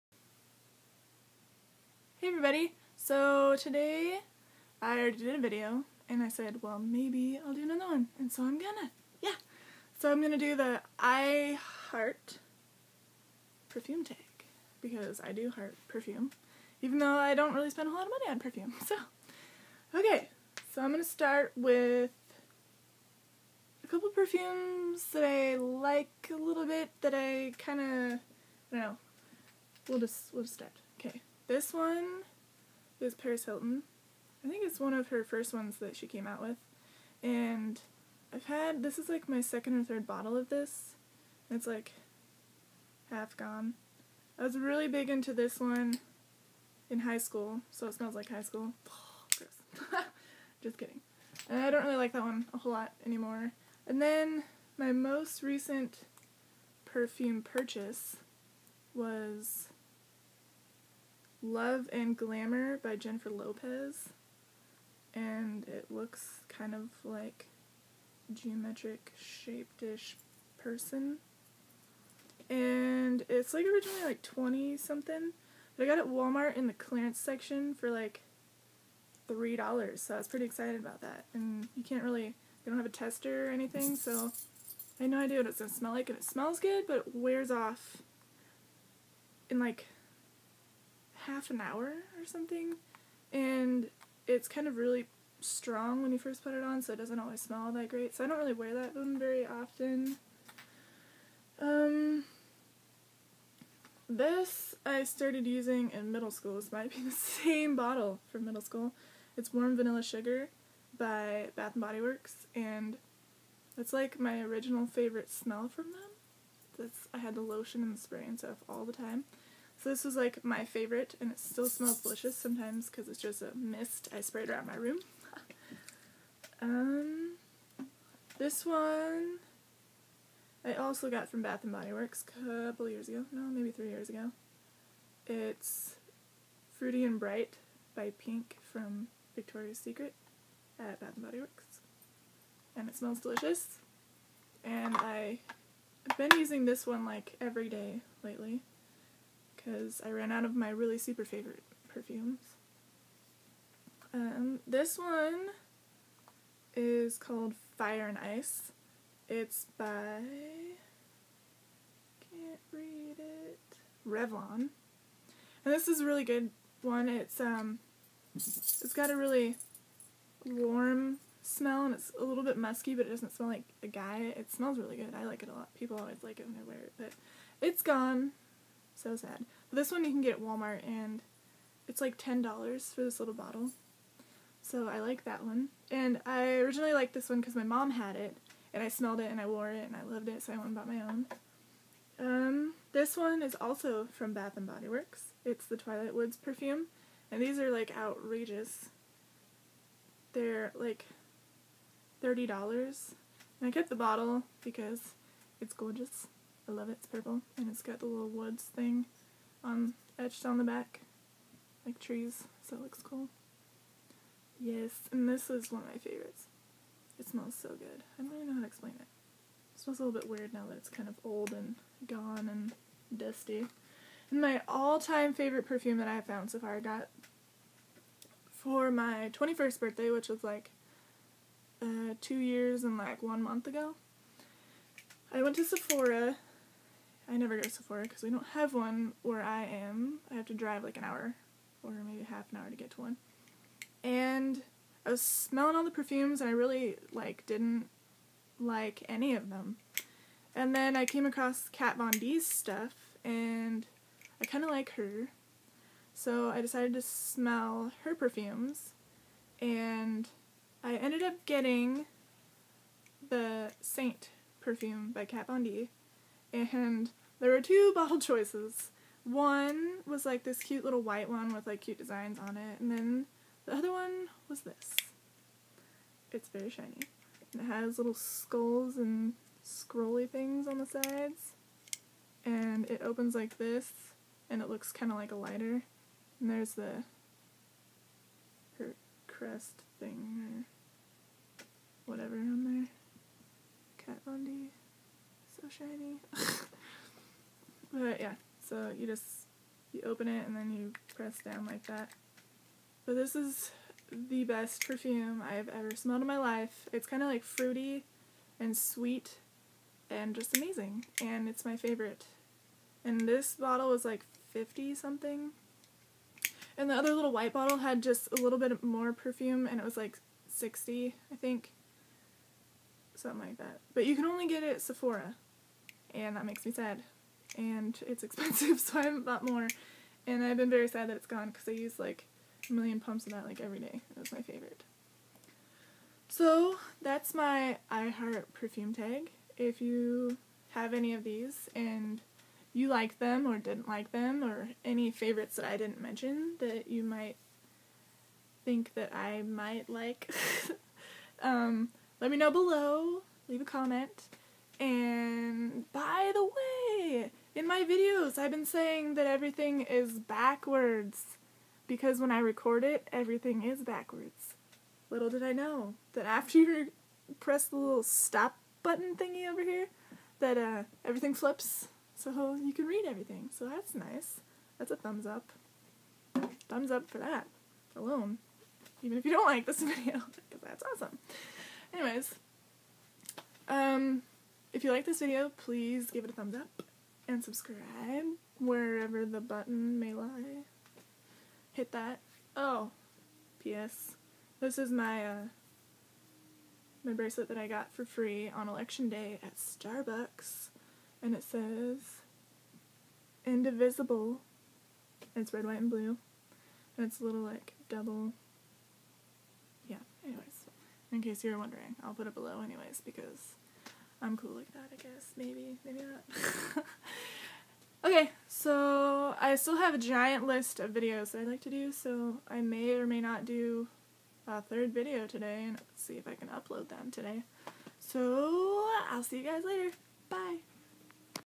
TAG: "I Heart Perfume"!
So...totally spaced looking for the actual questions for this TAG.. Whoops! Hahaha
Who doesn't like perfume?? Haha
Smelling good is awesome!

1. Paris Hilton
2. Love and Glamour by Jennifer Lopez
3. Warm Vanilla Sugar by Bath & Body Works
4. Fire & Ice by Revlon
5. Fruity & Bright

And as always, follow me in all these other awesome places!